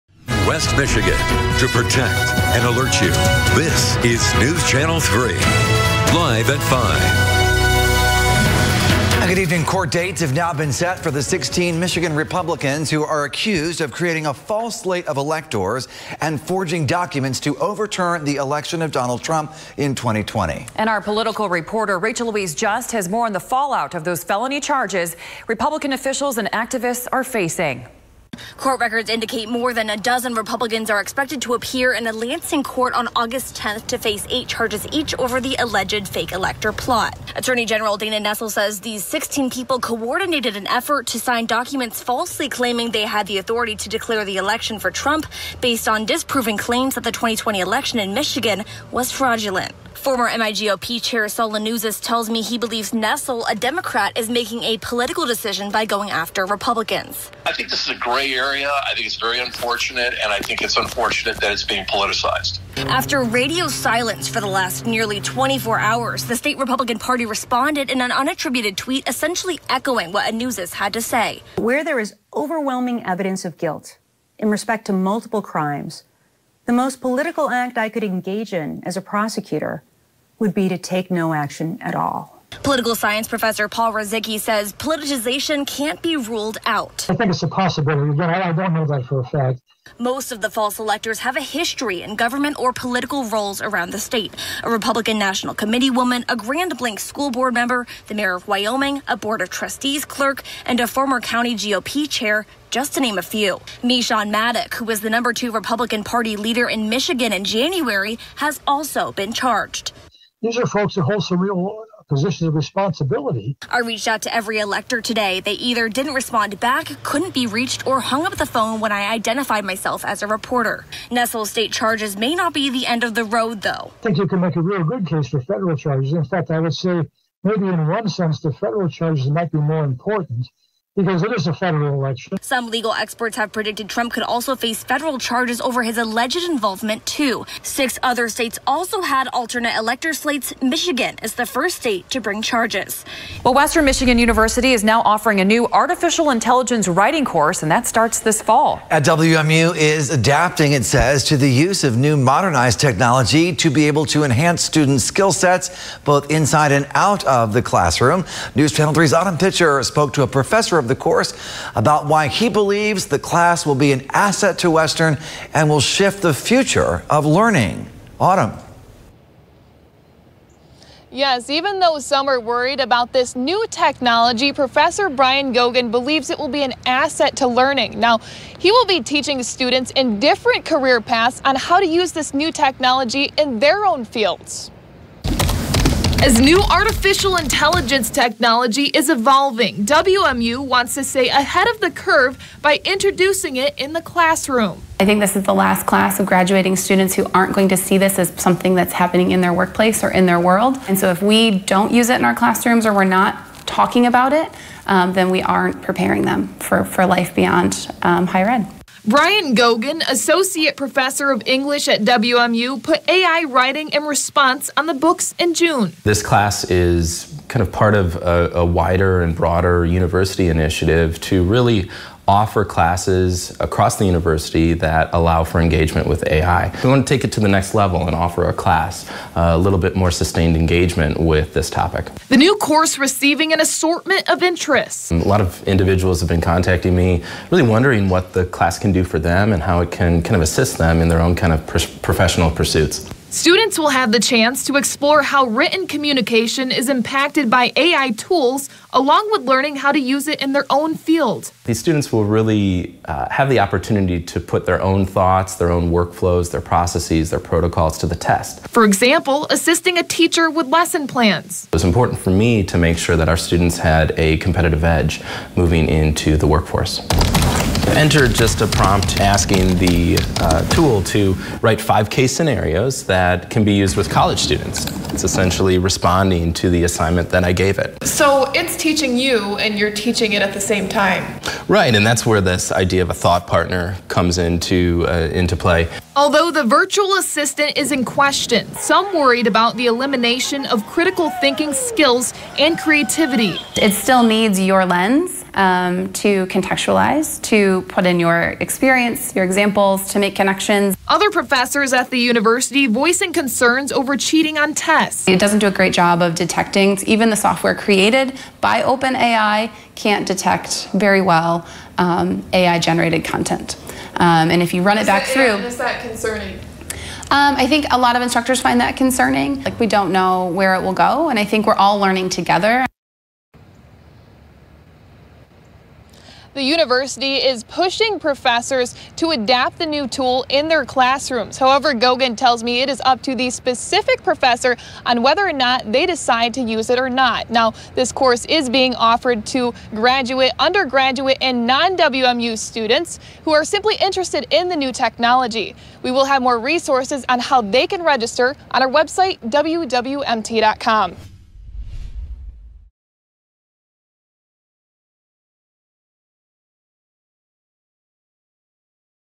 16 Michigan Republicans accused of creating a false slate of electorsLansing
Court dates have now been set for the 16 Michigan Republicans accused of creating a false slate of electors and forging documents to overturn the 2020 election in favor of Donald Trump.

More than a dozen Republicans are expected to appear in the 54A District Court in Lansing on Aug. 10 and 8:30 a.m. to face eight felony charges each over the alleged fake elector plot, according to state court records.

Plus, WMU launches AI writing course: A step towards modernized education

0:00 Court dates set for GOP 'fake electors' as Republicans argue politicization
2:23 WMU launches AI writing course: A step towards modernized education